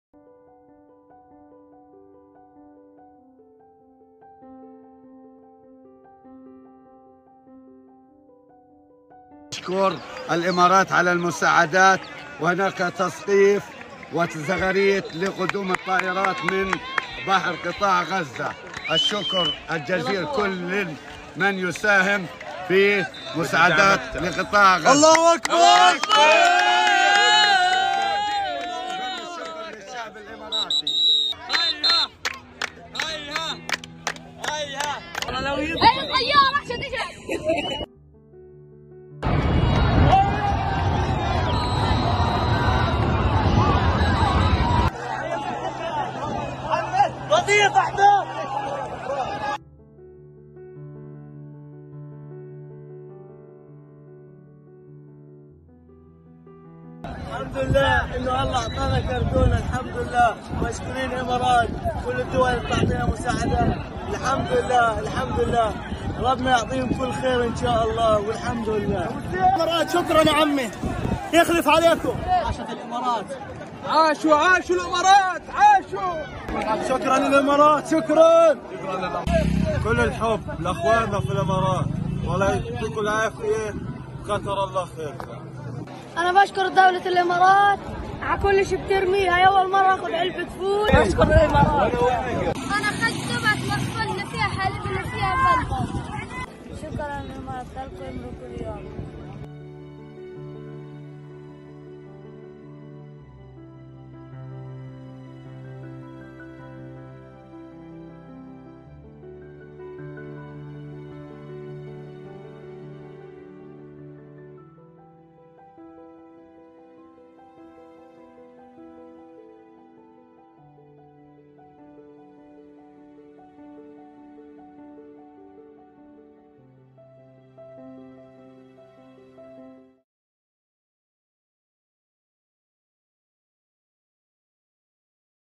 “طيور الخير” الإماراتية تتجاوز 1500 طن مساعدات لغزة
رسالة شكر من غزة لدولة الإمارات.. "طيور الخير" الإماراتية تتجاوز 1500 طن مساعدات عبر 27 إسقاطا جويا منذ بدء العملية الإنسانية لدعم الشعب الفلسطيني